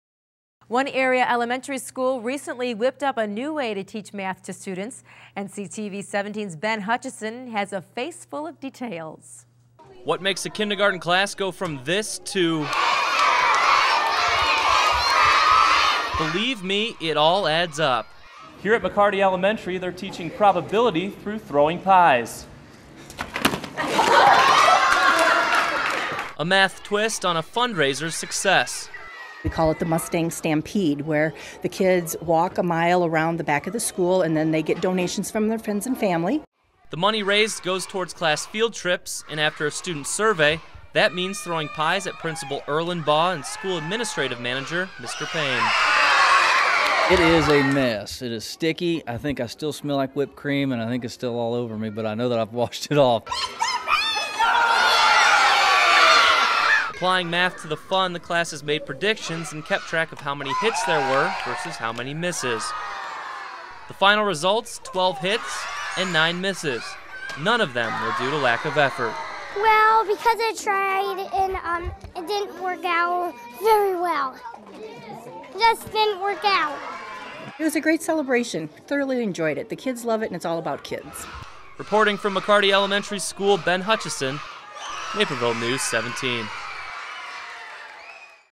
McCarty Pies
One area elementary school recently whipped up a new way to teach math to students.  NCTV17's Ben Hutchison has a face full of details.